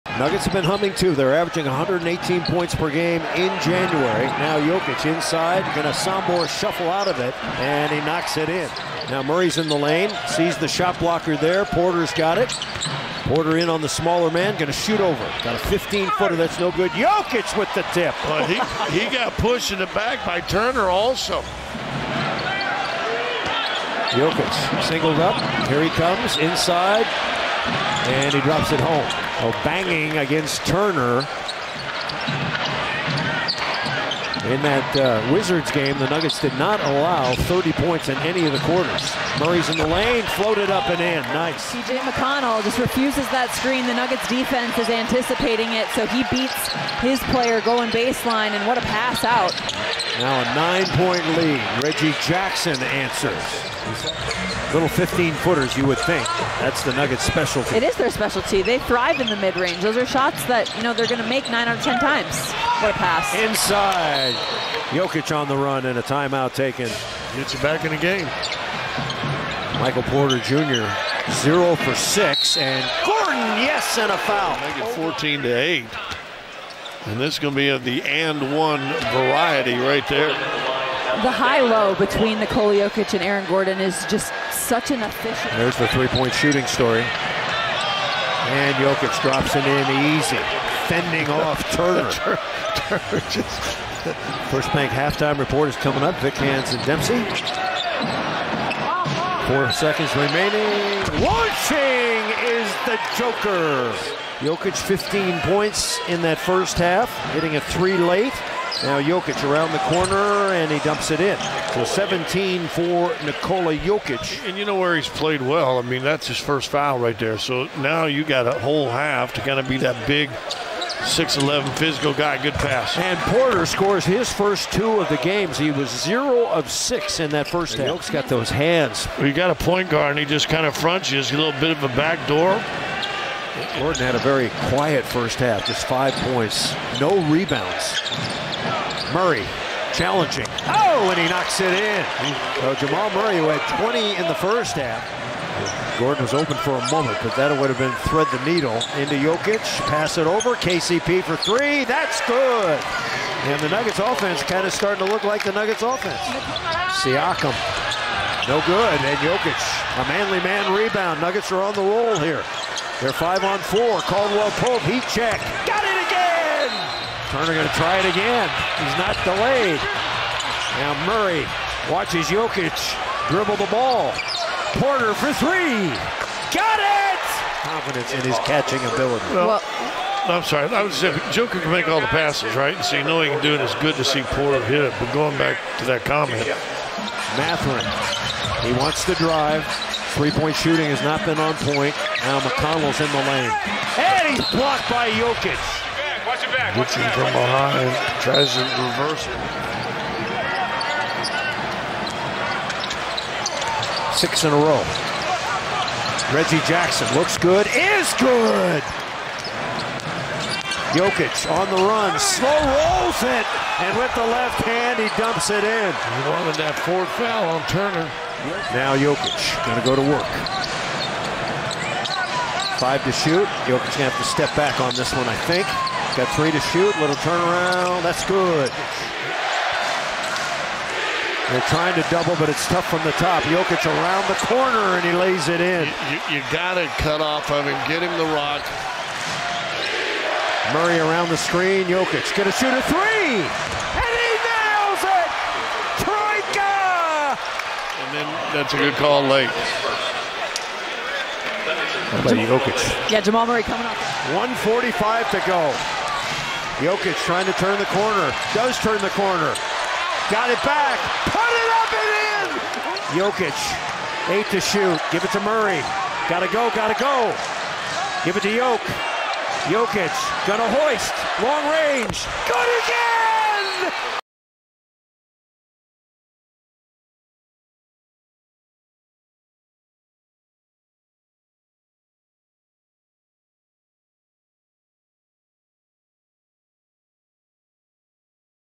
The TRIPLE-DOUBLE KING 👑 31 points, 13 rebounds & 10 assists for Jokic! | Top Plays v Indiana Pacers
Nikola Jokic recorded the 118th triple-double of his career last night as the Denver Nuggets went up against the Indiana Pacers.